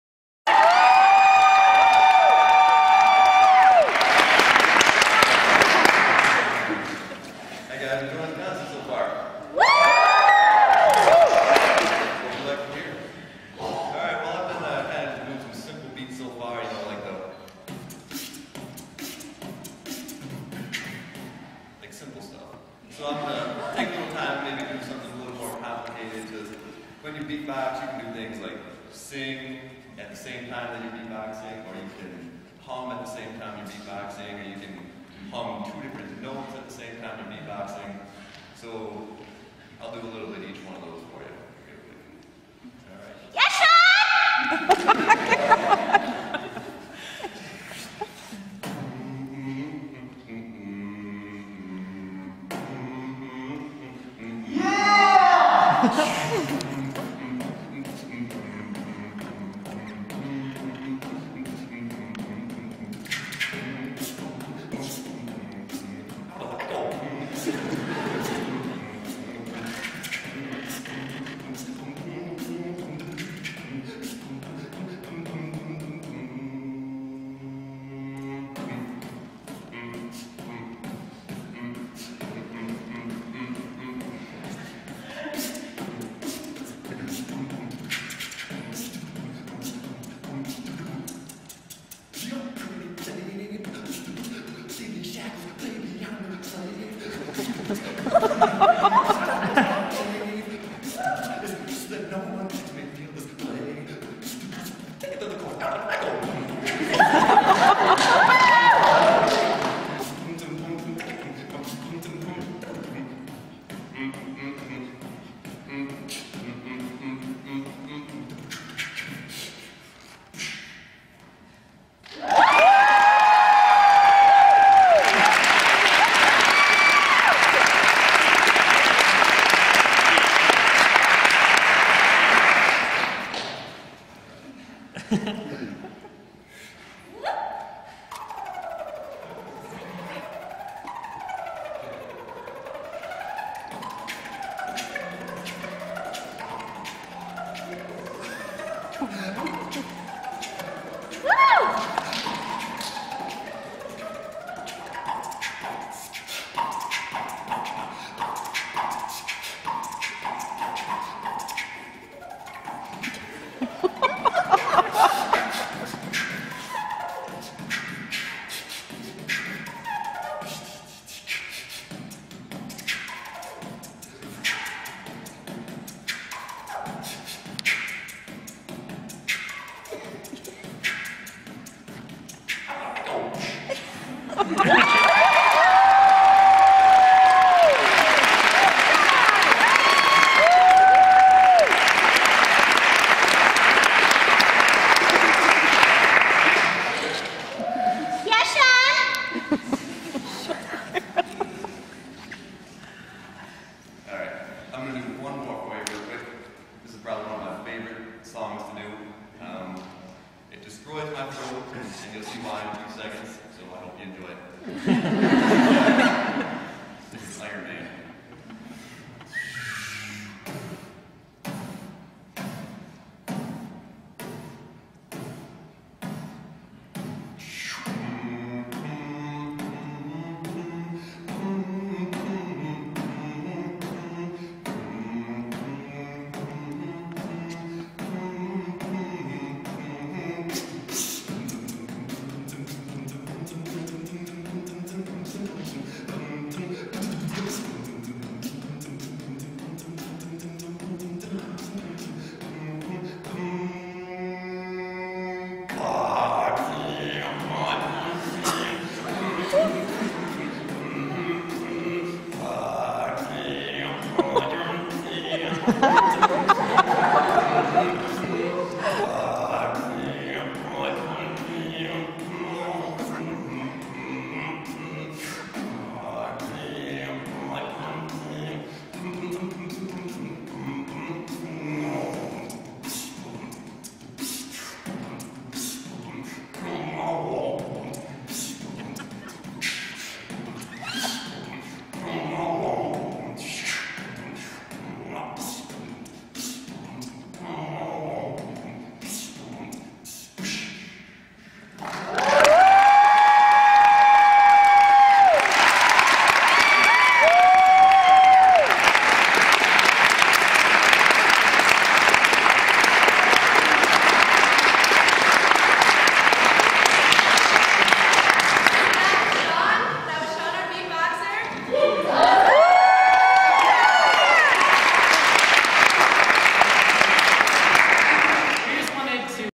Sean Connelly Beatbox
GV GrooVe!'s Beatboxer, Sean Connelly, lays down a beat for the audience to understand how GrooVe-y we are at the Spring 2010 concert. April 16th, 2010.